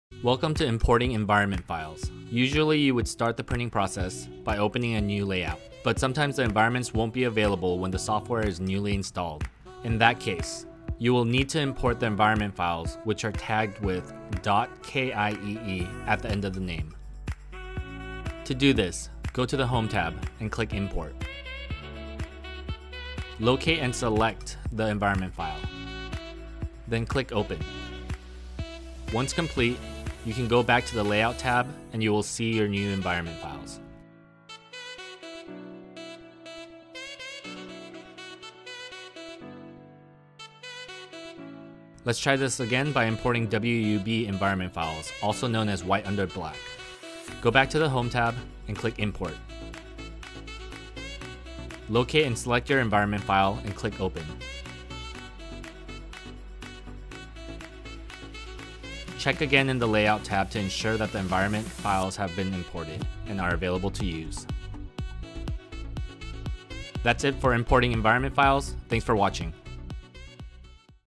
Kothari NeoRip F2100 - Importing Environments Guide
Learn how to import environment files for Kothari NeoRip.

Video Chapters
Import Environment Files (.kiee) (0:00)
Check Environment Files (0:28)
Import WUB Environment Files (0:41)
Check WUB Environment Files (0:58)